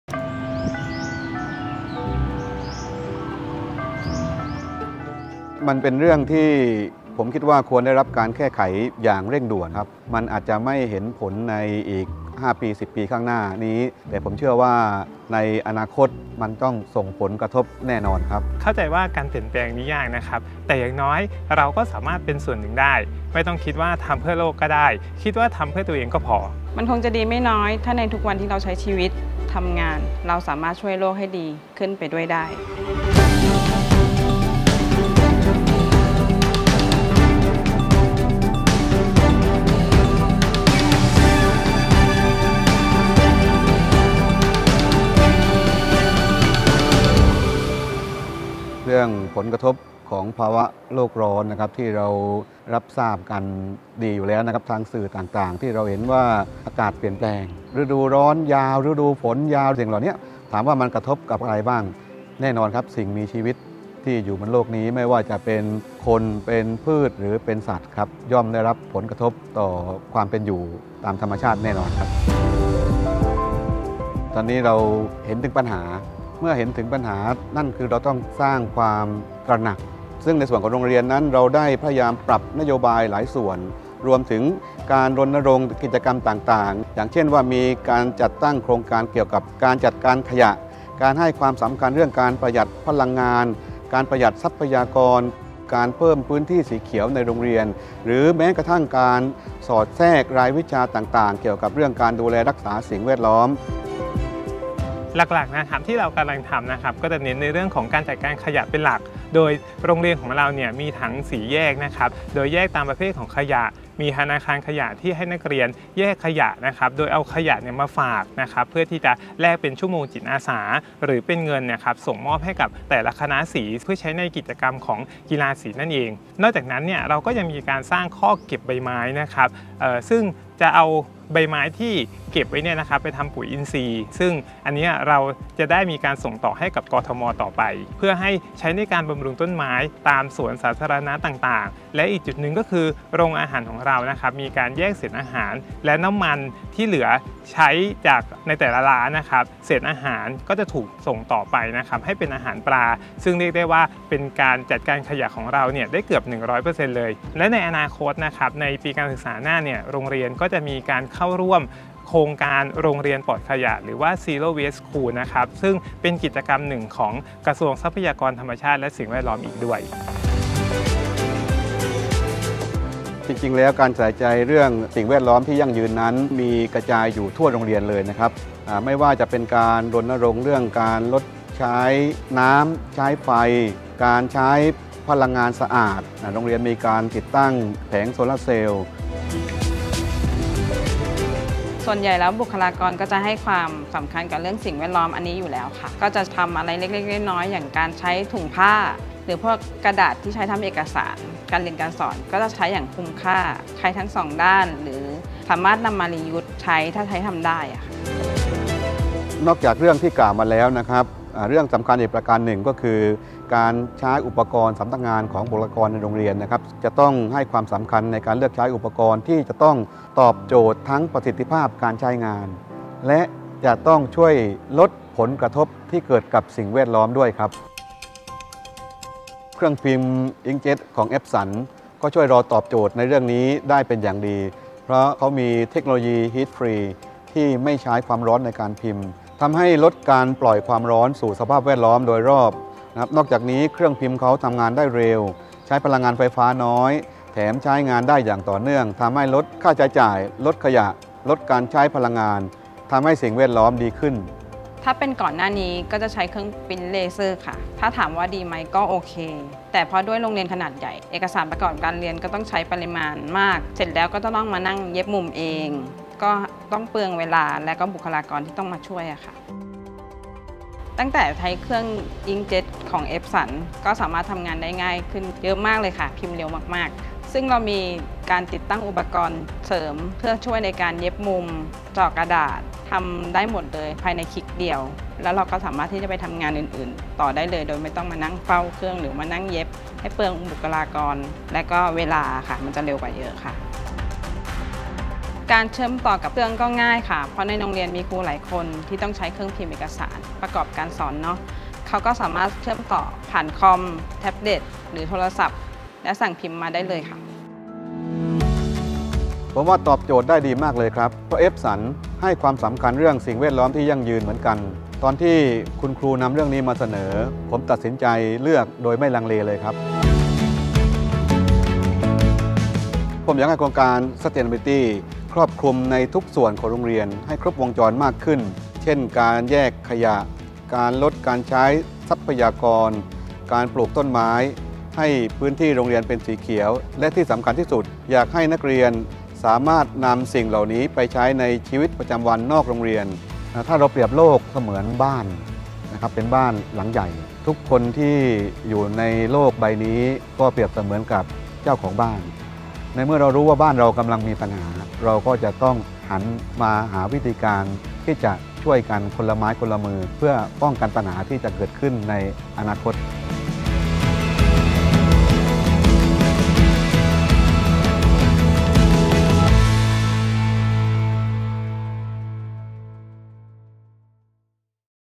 Cerita Pelanggan: Bodindecha Sing Singhaseni School & Printer Inkjet Bisnis Epson
Setiap orang memiliki peran mereka untuk keberlanjutan. Dalam video ini, Sekolah Bodindecha, Thailand, menjelaskan upaya bersama mereka untuk kelestarian lingkungan di seluruh sekolah. Tonton video ini untuk mengetahui bagaimana pelanggan membuat keputusan untuk membeli printer Epson BIJ WorkForce untuk manfaat keberlanjutan, efisiensi kerja, dan kenyamanan yang diberikan printer ini.